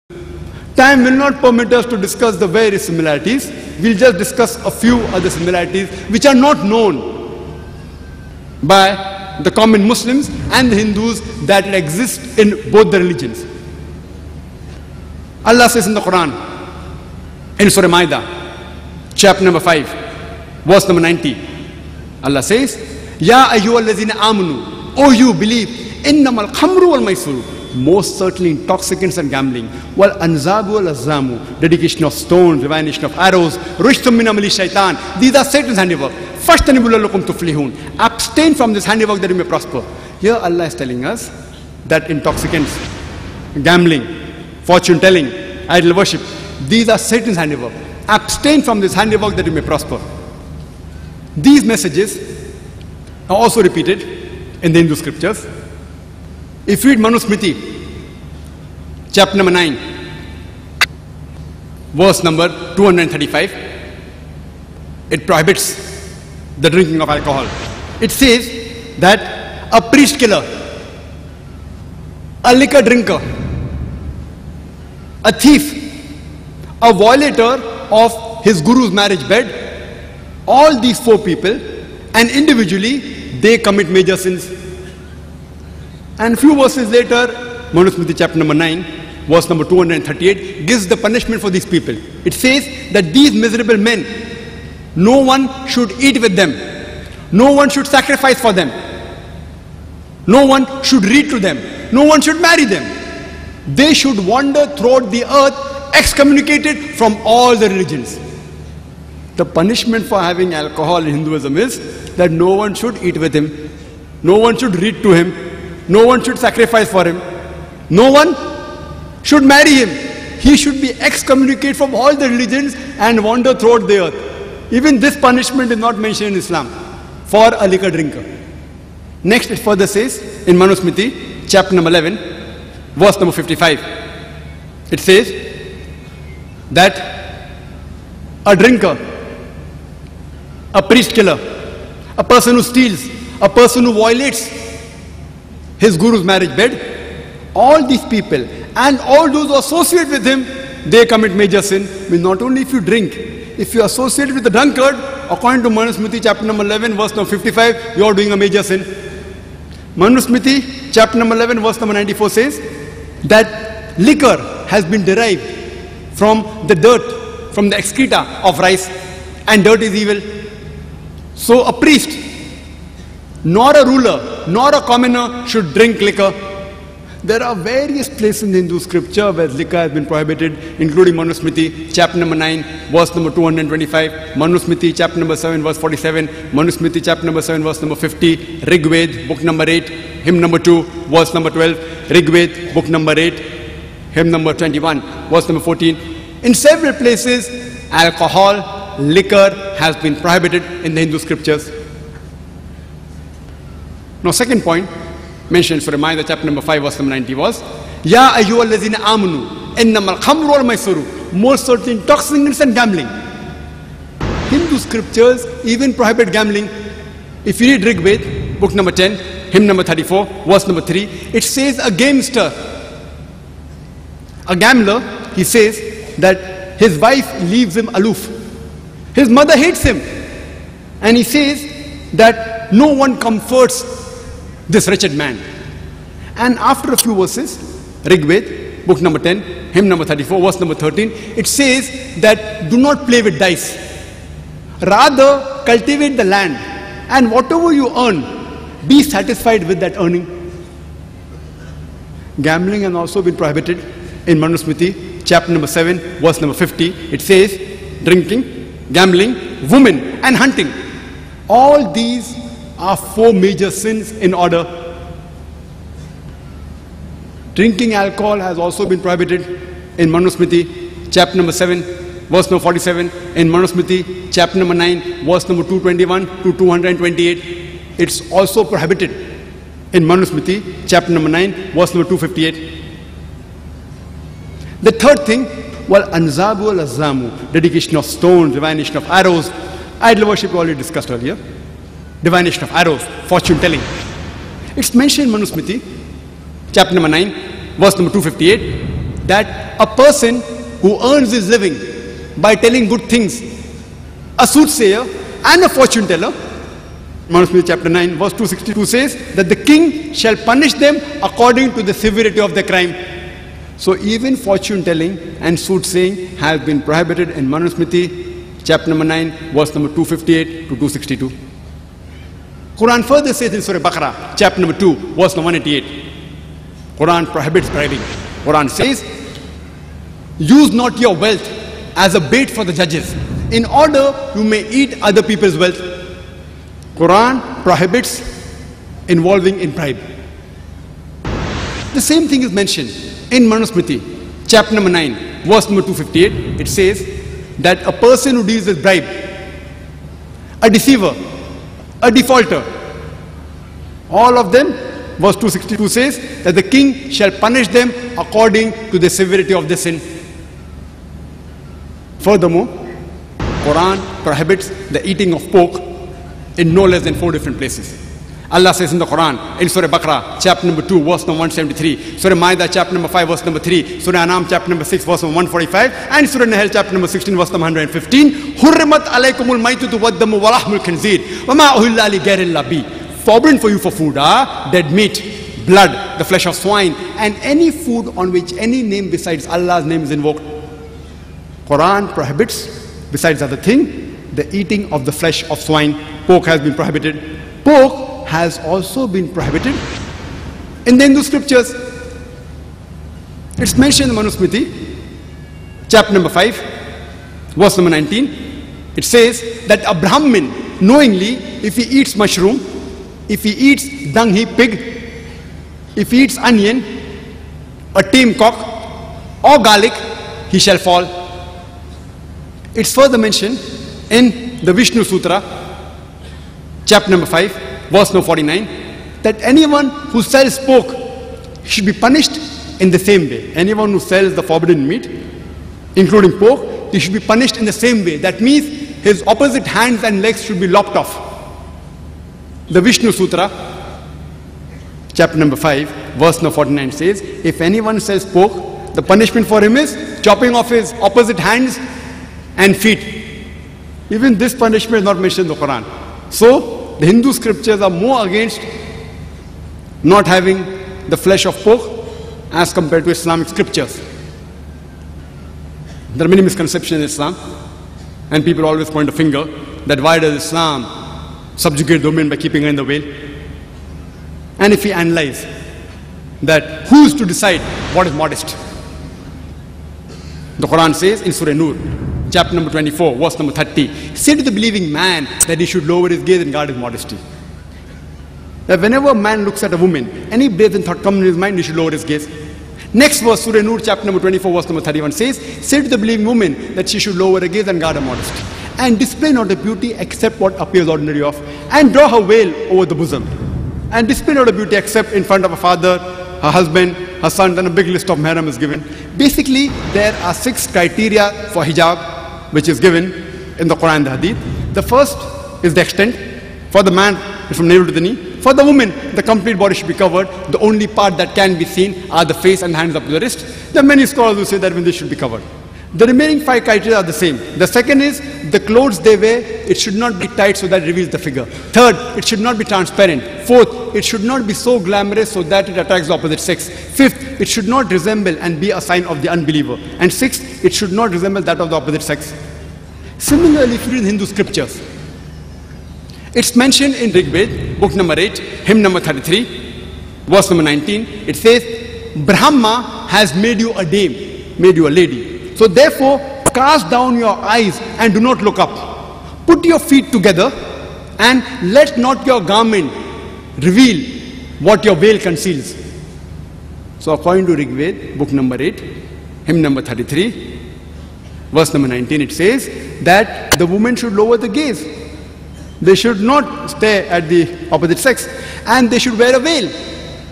Dr.Zakir-Similarities between Hinduism and Islam 11 of 19
Dr. Zakir Naik points out the similarities between Islam and Hinduism from both the Holy Scriptures of the two religions The Quran and the Vedas and Puranas